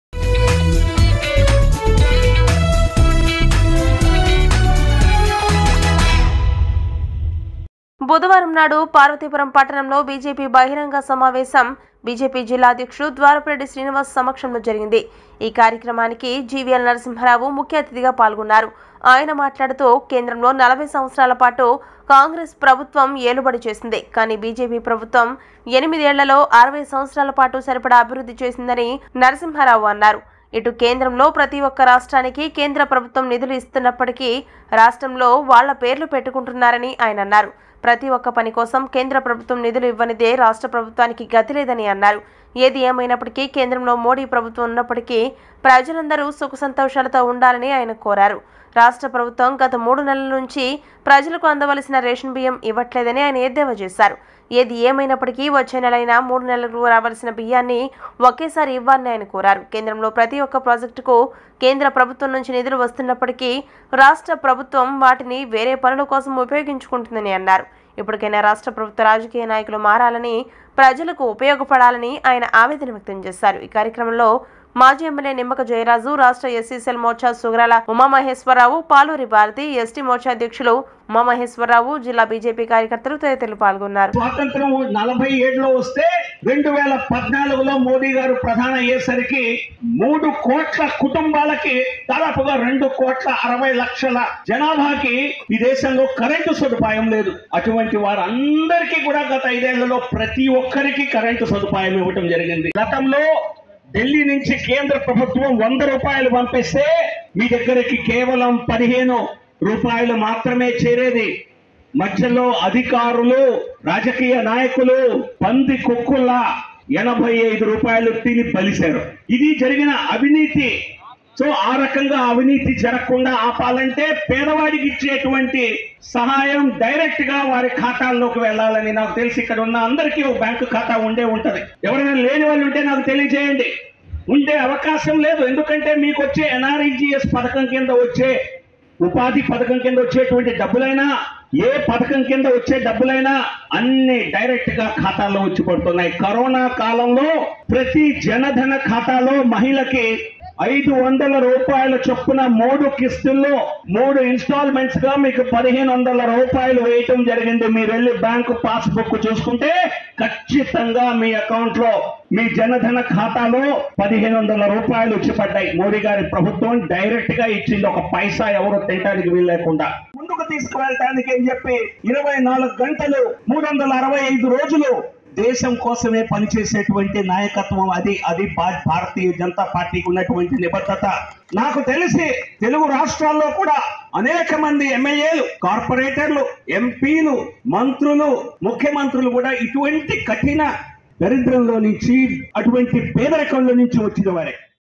BJP ప్రభుత్వం ఎనిమిది ఏళ్లలో 60 సంవత్సరాల పాటు సరిపడా అభివృద్ధి చేసింది: GVL నరసింహారావు..
@truthnewstelugu BJP ప్రభుత్వం ఎనిమిది ఏళ్లలో 60 సంవత్సరాల పాటు సరిపడా అభివృద్ధి చేసింది: GVL నరసింహారావు..